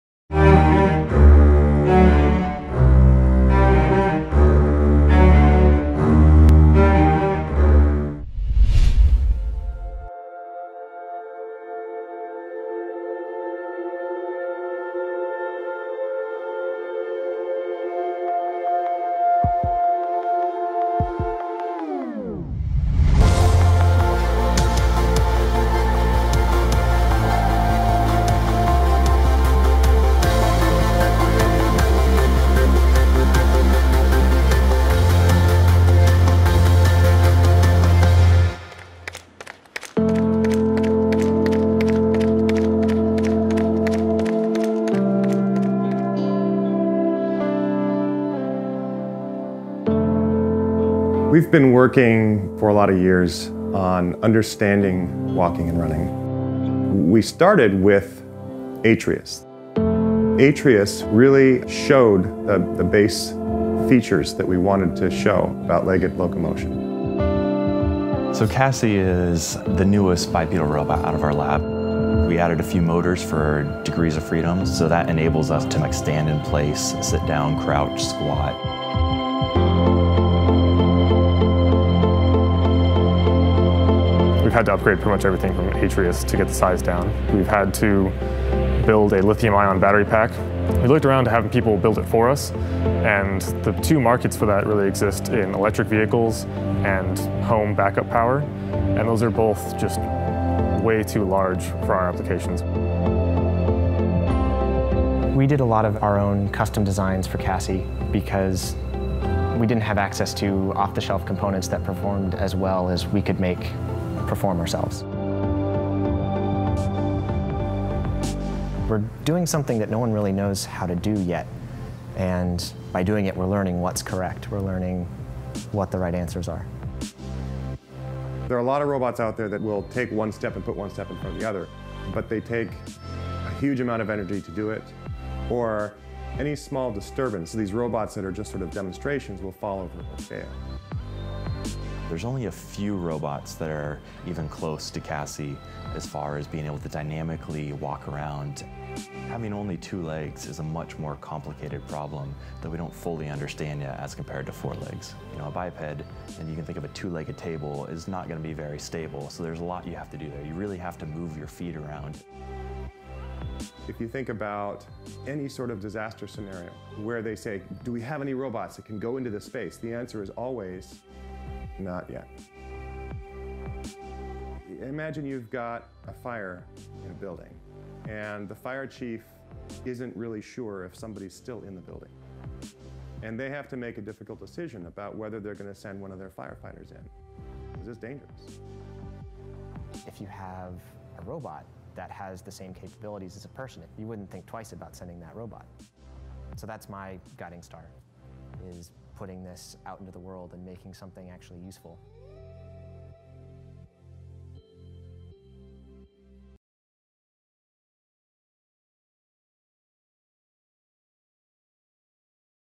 Двуногий робот Agility Robotics Cassie (Robotics.ua)
Инженеры компании Agility Robotics разработали двуногого шагающего робота Cassie. Отличительной особенностью данного механизма являются его ноги, разработанные на основе конечностей страуса. Такой дизайн позволяет роботу ходить по сложным неровным местностям и работать в качестве курьера.

Видео с Oregon State University